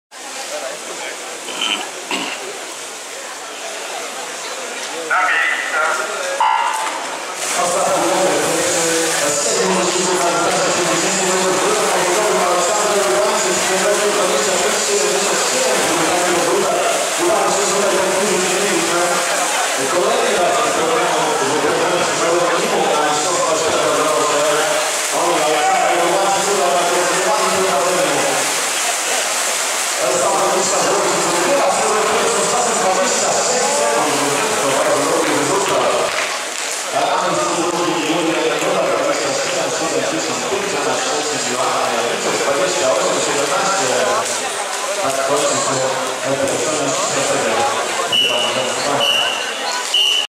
50 motylkowy kobiet 1 seria Grand Prix Puchar Polski Krakow 25-26.10.2014
1 Sieńczyk Jowita SK 97 Żak Biala Podlaska 28.05
2 Peda Paulina 98 AZS AWF Katowice 27.76
3 Radlińska Marcelina 93 AZS AGH Kraków 27.70
4 Urbańczyk Aleksandra 87 AZS UŁ PŁ Łódź 25.58
5 Janiczek Joanna SO 96 Unia Oświęcim 27.52
6 Jagoda Julia 95 AZS AWF Katowice 27.73
7 Szczepanik Joanna SO 96 Unia Oświęcim 28.04
8 Stasinowska Katarzyna 94 AZS UŁ PŁ Łódź 28.33